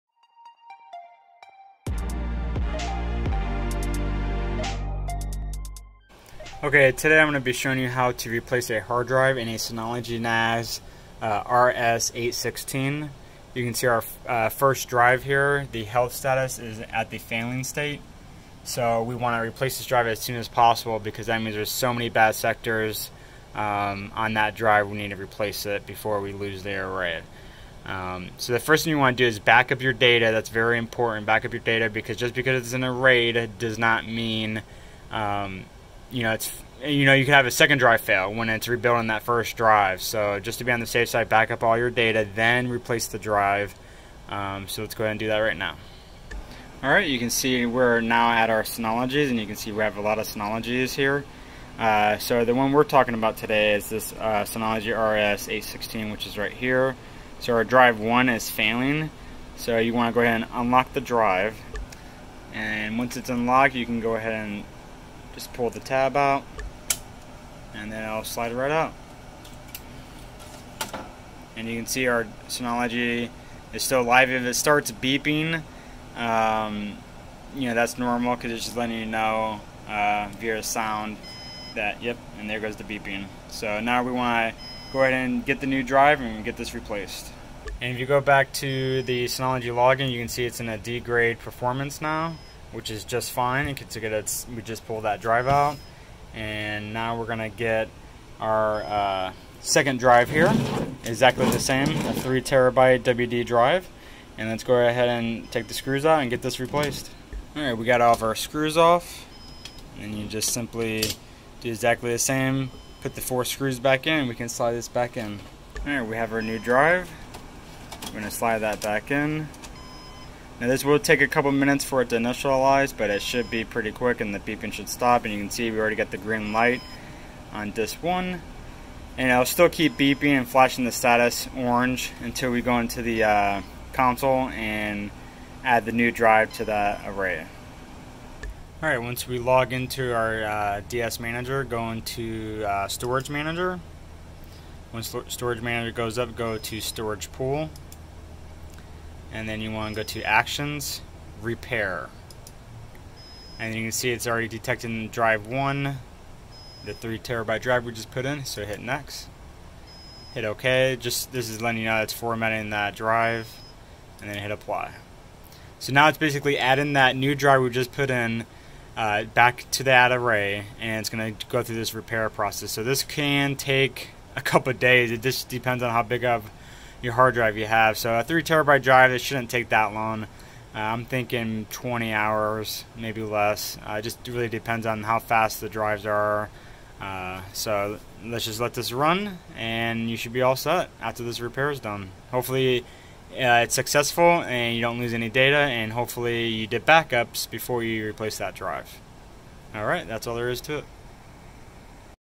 How to replace Failed drive in a Synology NAS RS816
In this video I will show you how to replace a hard drive in a Synology NAS RS816 but this can be done in really any model of Synology. Steps will be the space just hard drive might be in a different area of the NAS. Warning please backup the data on your NAS before you do this as you can get another failed drive and lose the whole array and all your date.

Synology 8 Bay NAS
Synology 4 Bay NAS RS819
Synology M.2 Adapter Card